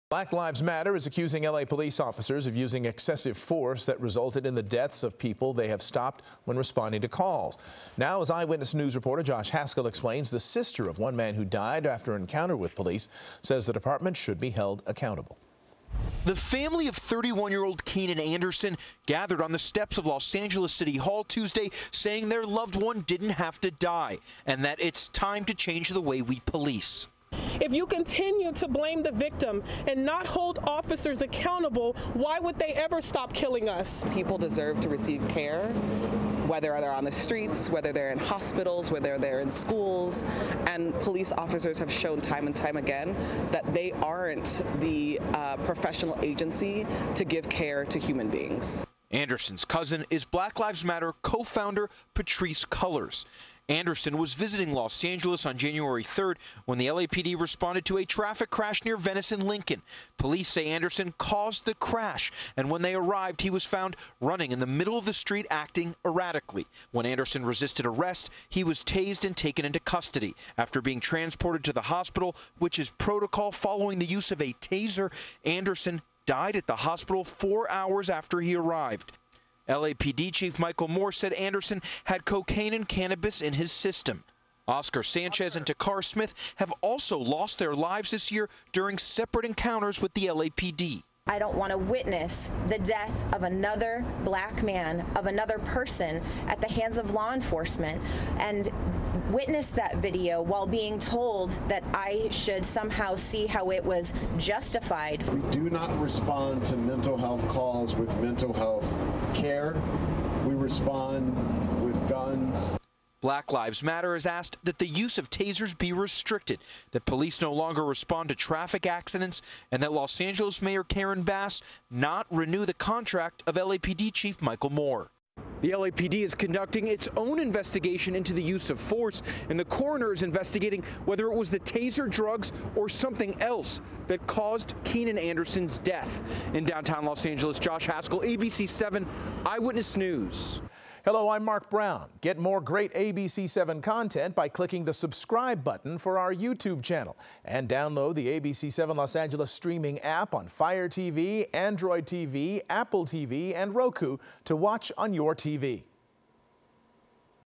Keenan Anderson case: Death of teacher after Taser incident prompts calls for LAPD reform
The family of Keenan Anderson, a 31-year-old teacher who died hours after he was repeatedly shocked with a Taser by police, gathered at City Hall to demand LAPD be held accountable. Full story